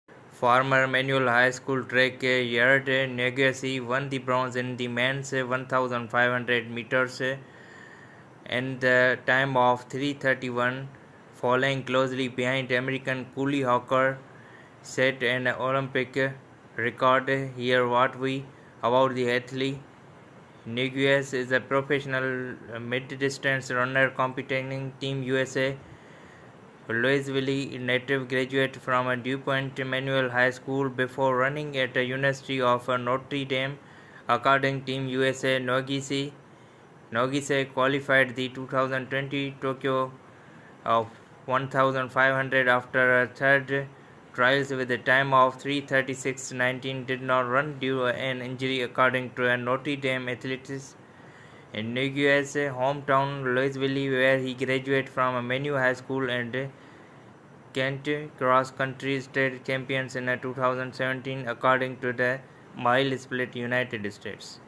Yared Nuguse gets Bronze 1500 meter's with a time of 3:31.72 behind American Cole Hocker
Yared Nuguse named  Bronze 1500-Meter Final